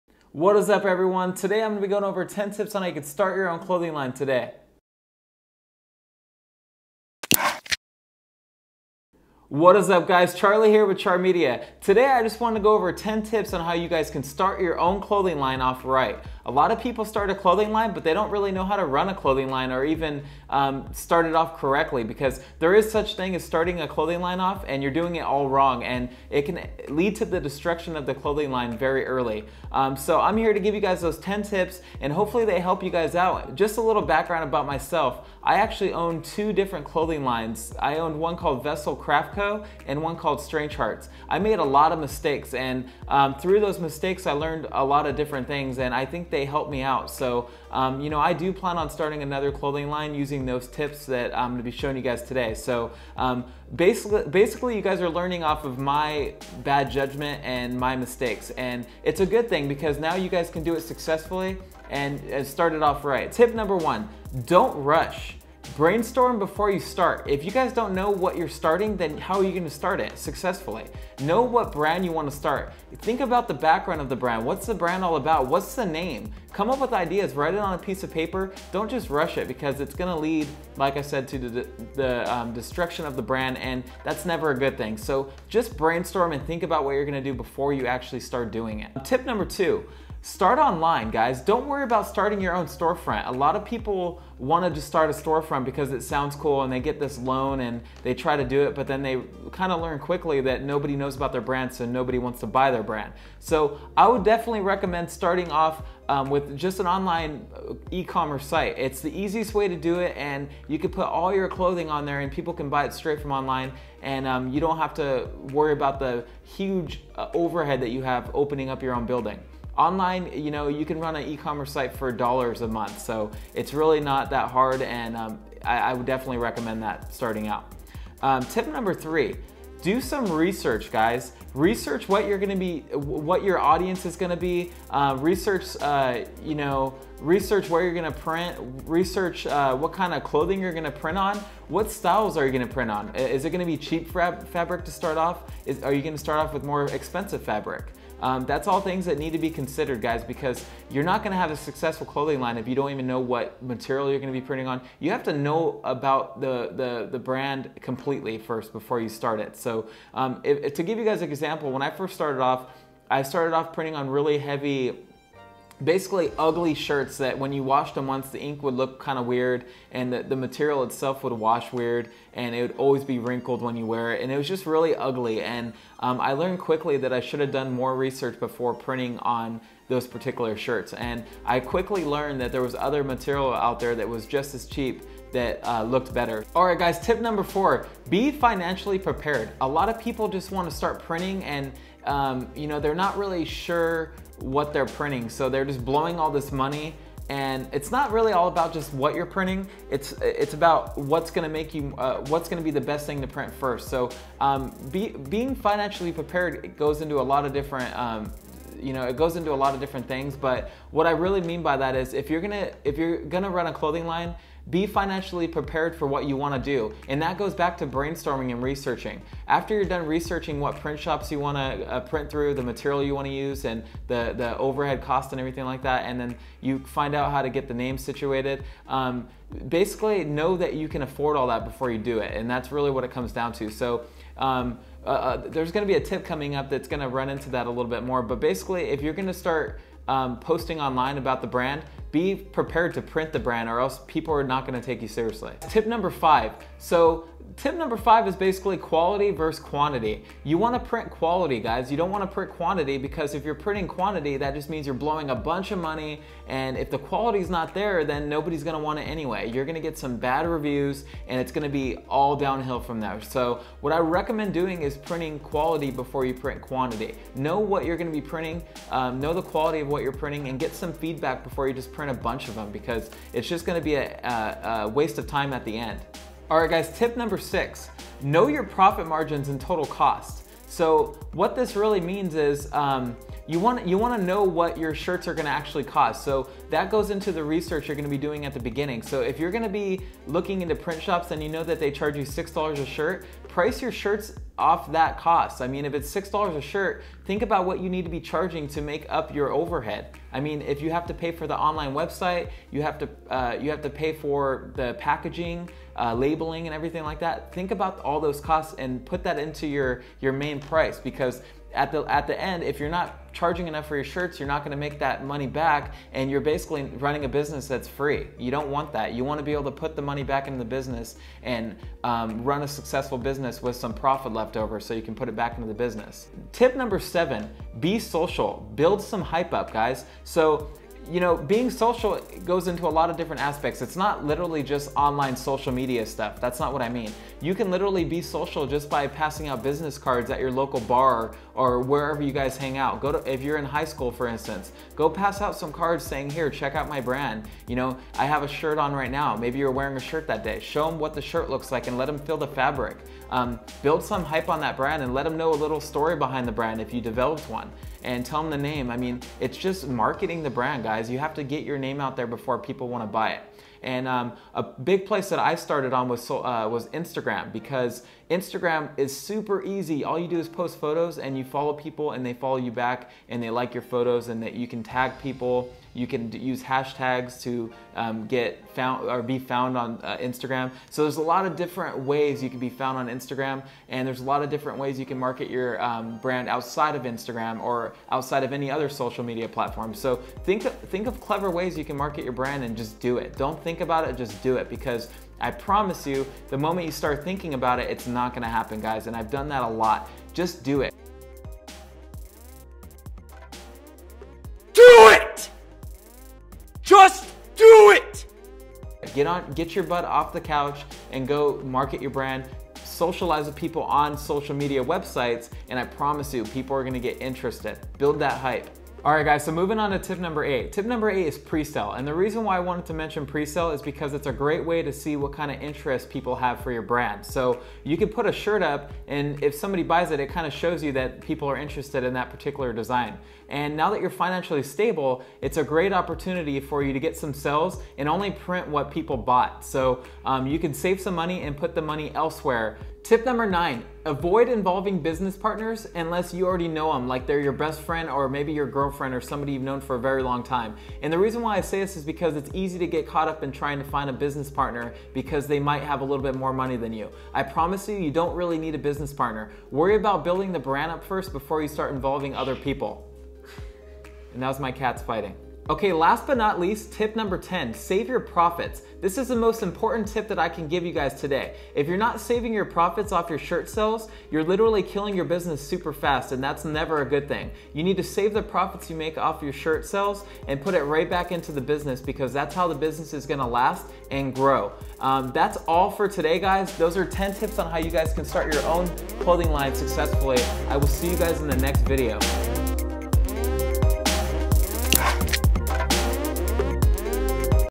10 Tips To Starting Your Own Clothing Brand From Your Couch
Tiktok you're looking to start a clothing brand in 2023 this is the video for you. I share some tips and industry secrets that helped me build a profitable brand from the ground up. Here are the 10 tips!